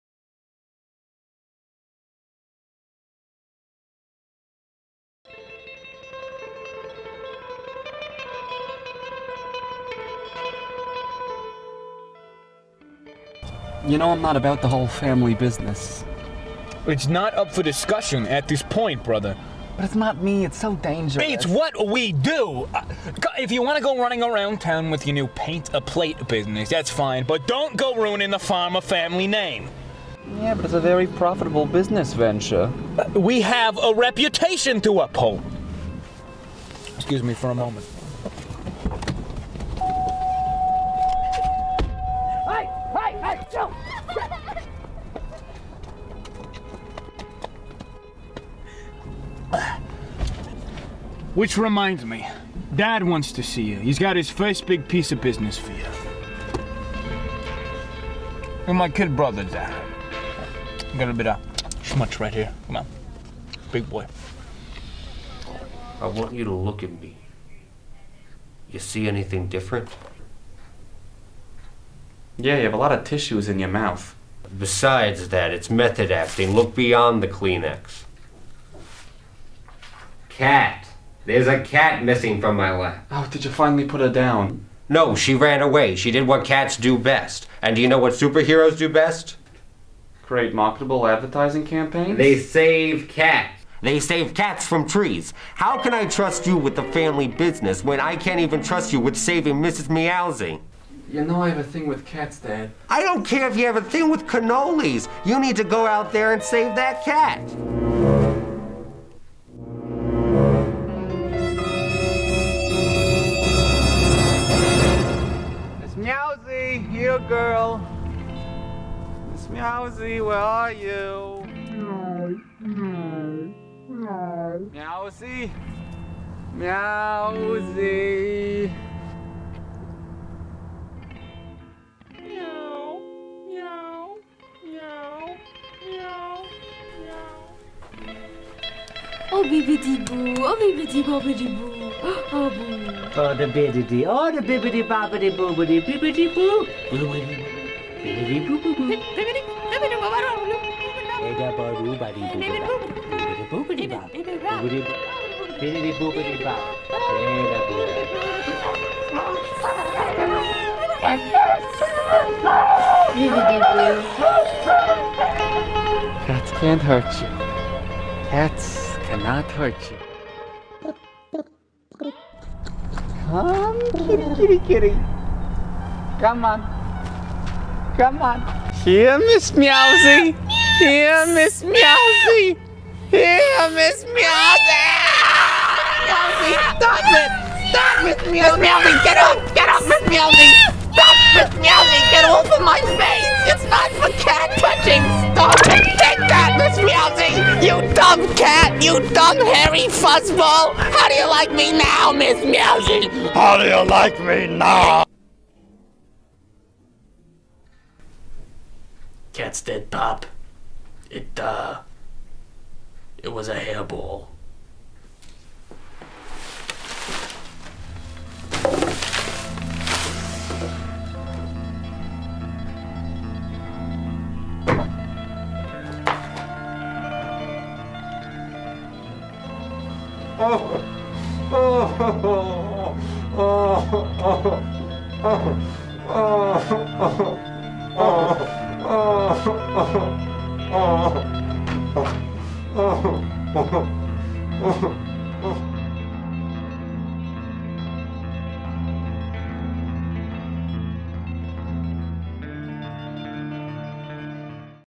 (NY 2015) 'The Godfarmer' Hauppauge HS
Winner - 10th Place, NEW YORK 2015!

Genre: Superhero
Character, backstory: Dara or Darren Farmer, has a serious phobia.
Prop: A plush toy (stuffed animal).
Line of Dialogue: I knew that was going to happen